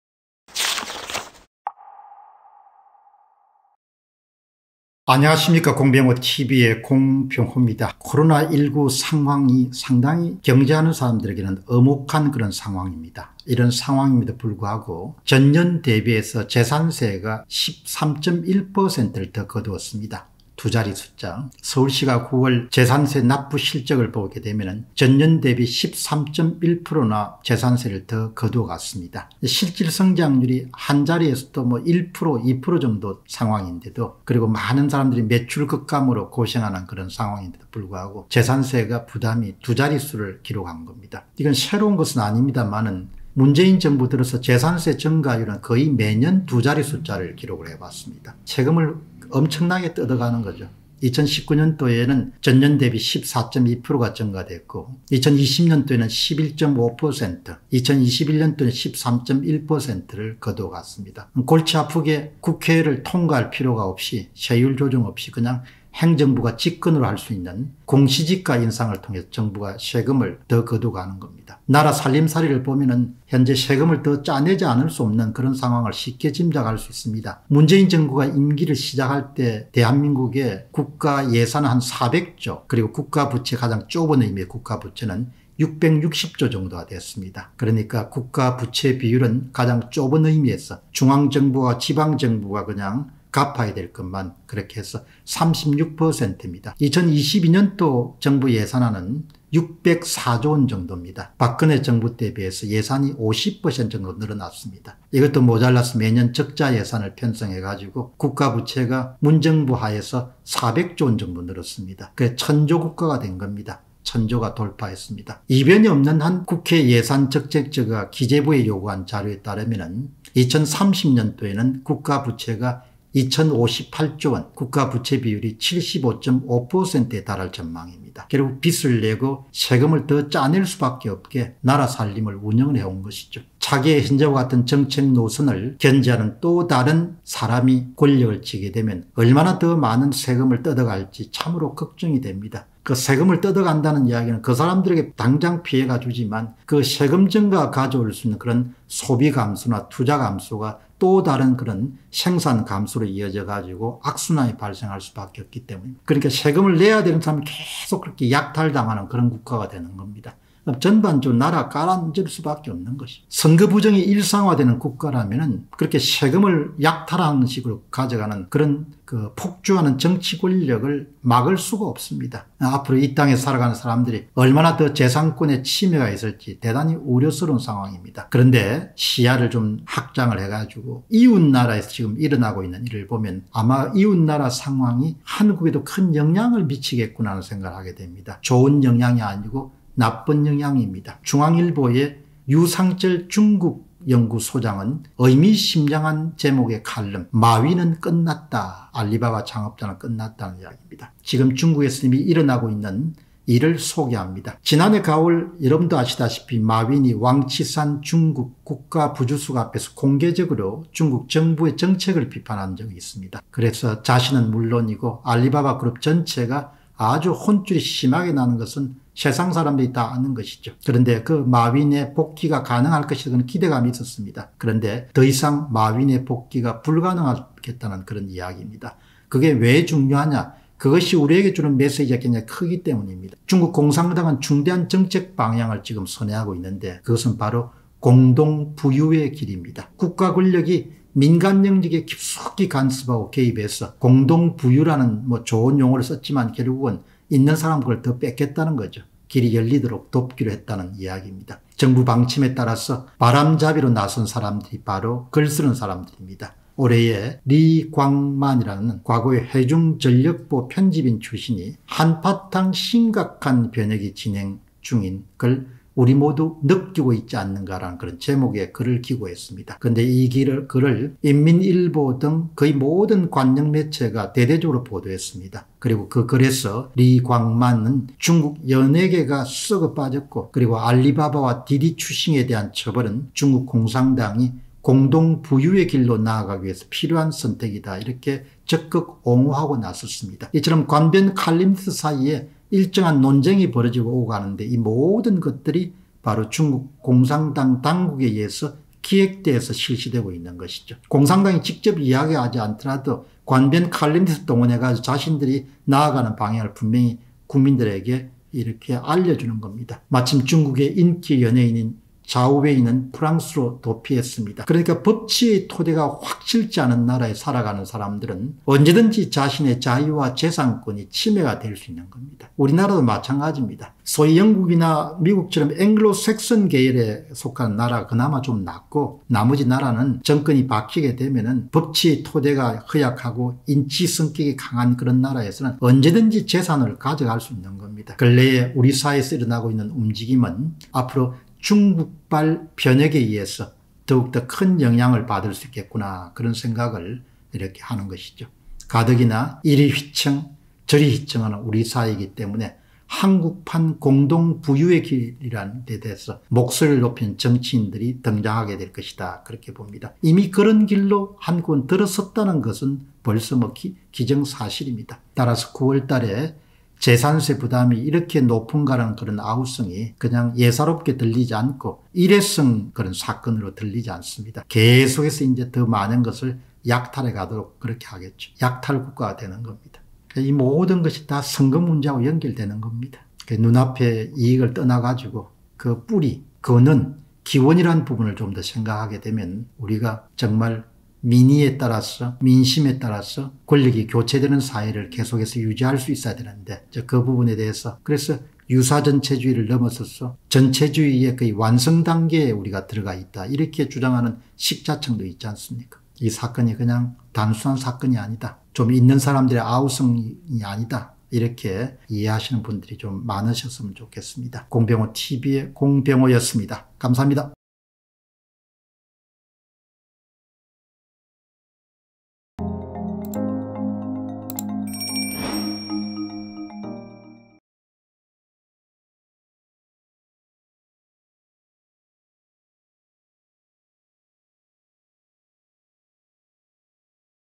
무슨 세금, 이리 많이 걷을까 / 서울시 재산세 부담 13.1% 증가 / 공시지가를 올리기만 하면 거둘 수 있는 세상 / 조세법정주의는 실종 / 옥죄는 일은 가속화 [공병호TV]
Bitcoin : 3MTW849AkP2ftLeSK2teZHMmzLu7Z83s9U